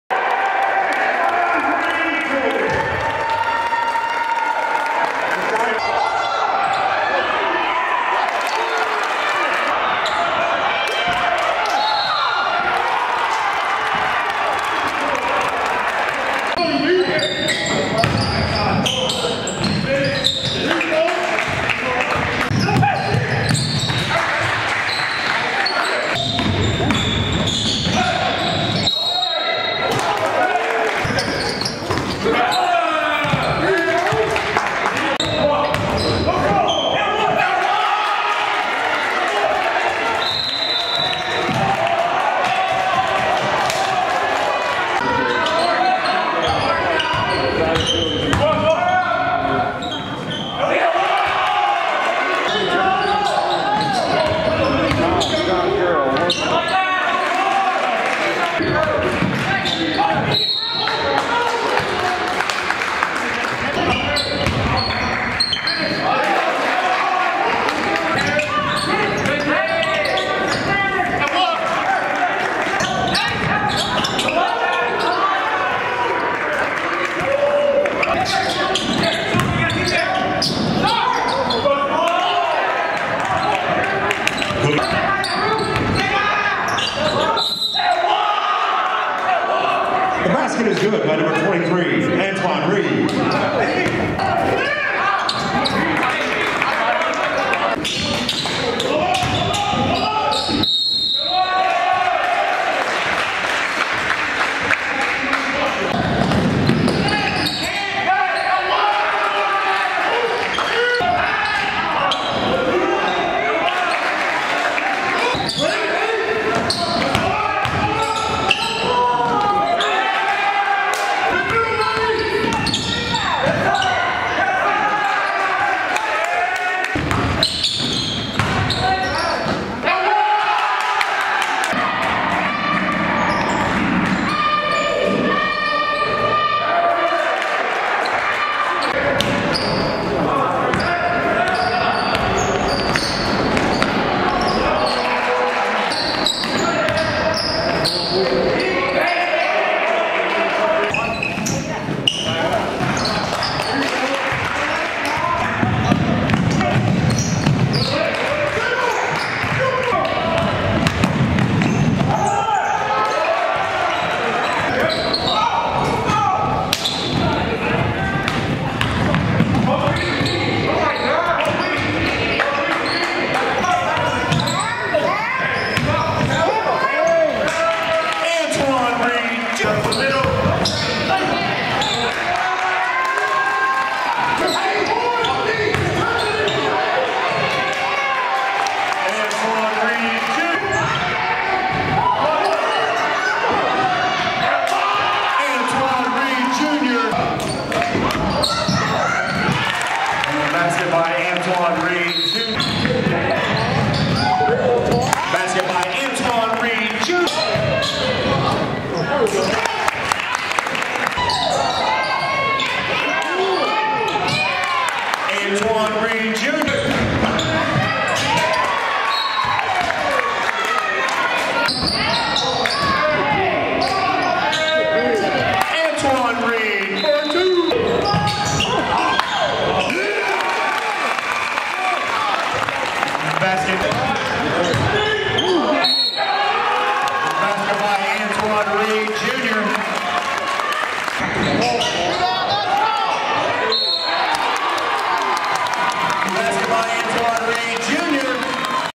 Antwoin Reed Jr '22 F Beechcroft High School Junior Season Highlights
Highlights from 2021 shorten season due to COVID